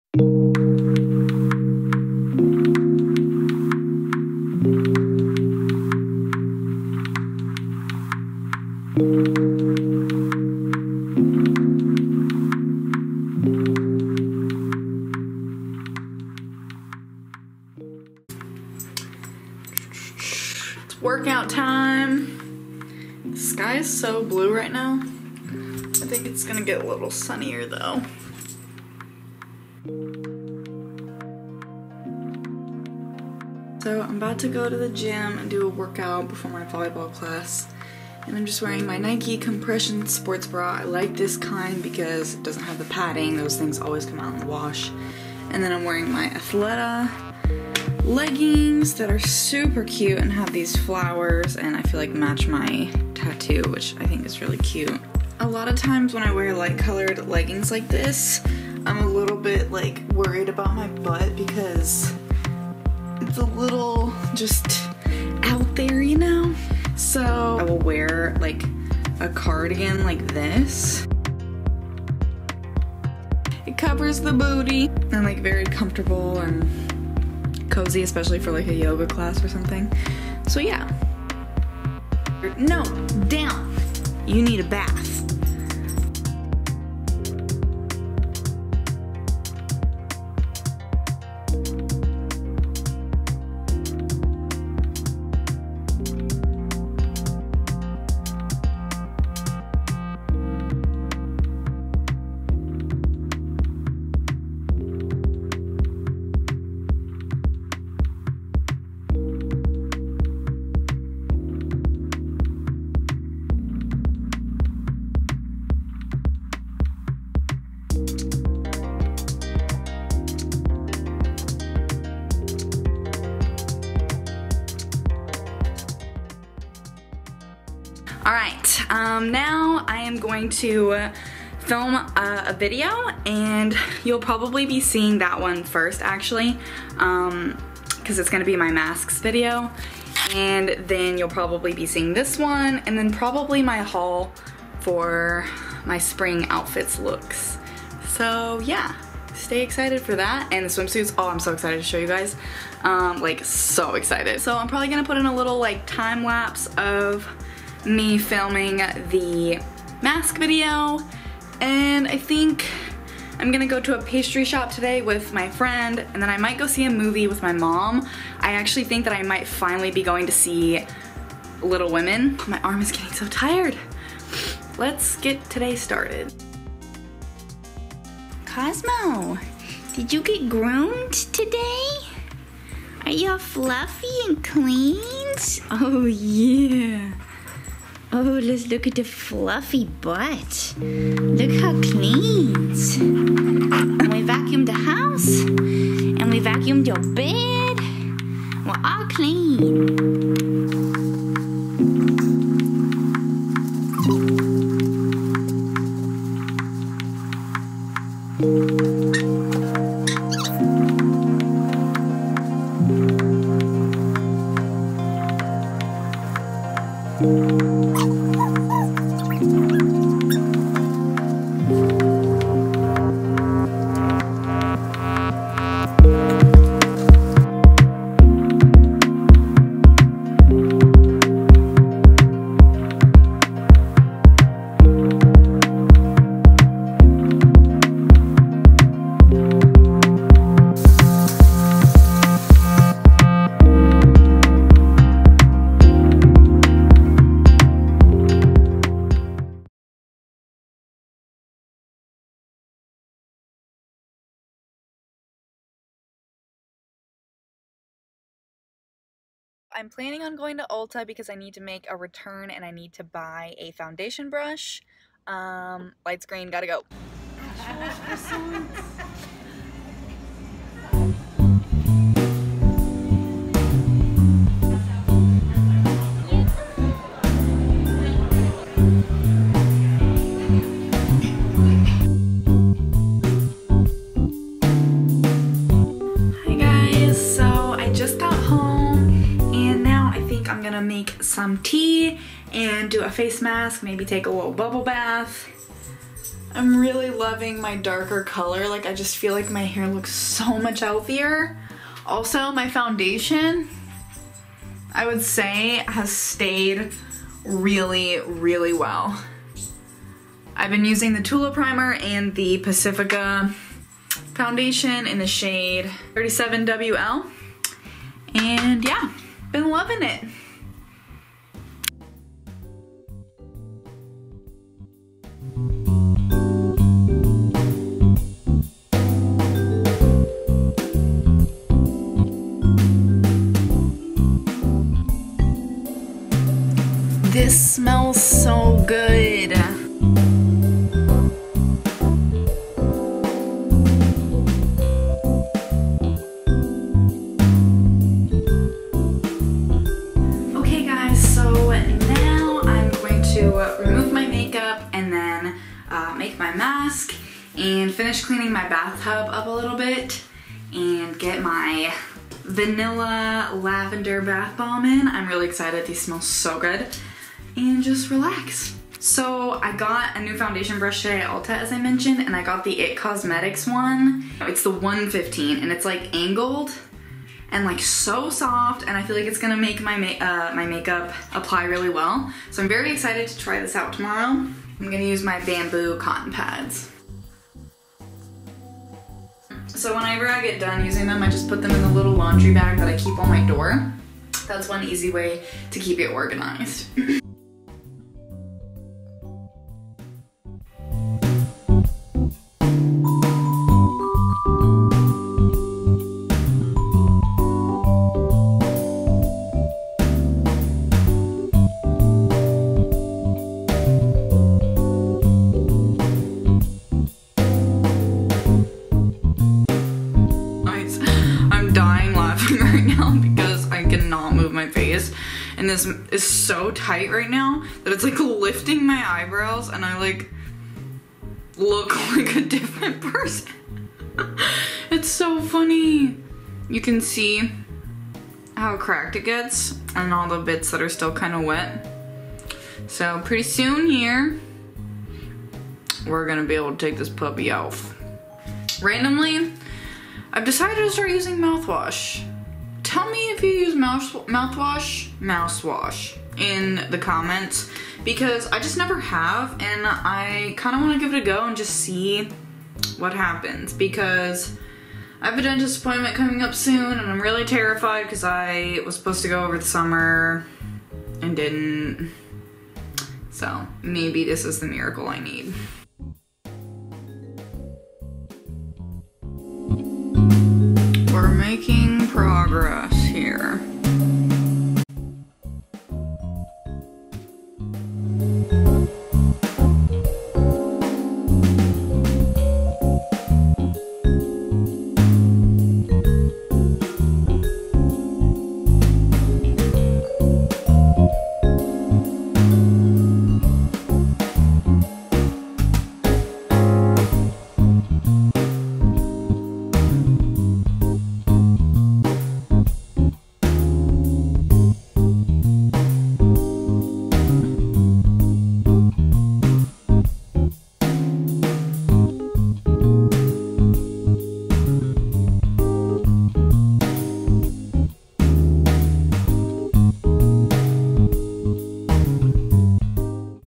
VLOG| Workout, Pastries, Bath Night (Pre-Pandemic)
SOOOO guys... I found all the footage I had a mental breakdown over losing back in February... after all the hard work I was wrecked... anyway I found it so I am posting them for your entertainment (:

Just remember the timeline is all off because ya know... the breakdown fiasco... so don't worry I am masking and doing all the things these days.

Stay safe out there peeps.